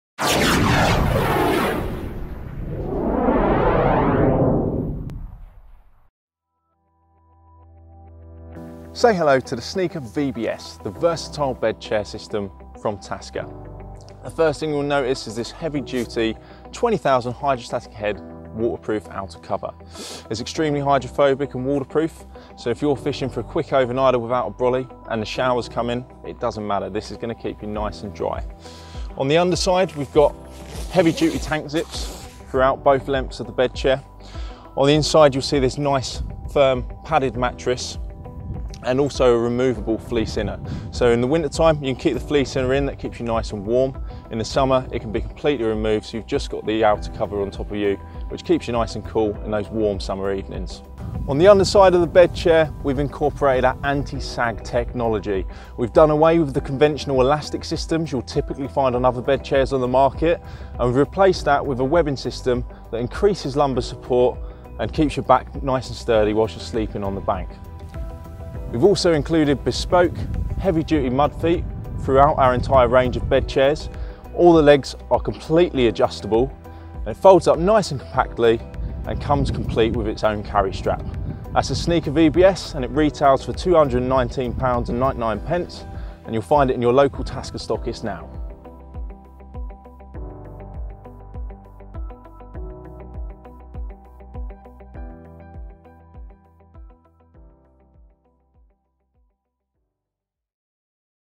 Taska TV: Sneeka VBS Bed System Explained - Carp Fishing Tackle
Ben Francis gives us an in-depth look at the new Sneeka VBS Bed System, and what features it has to offer and how you can get the best from it. If you're a short session angler that loves to be mobile when angling, we're sure that you will not be disappointed with the Sneeka VBS.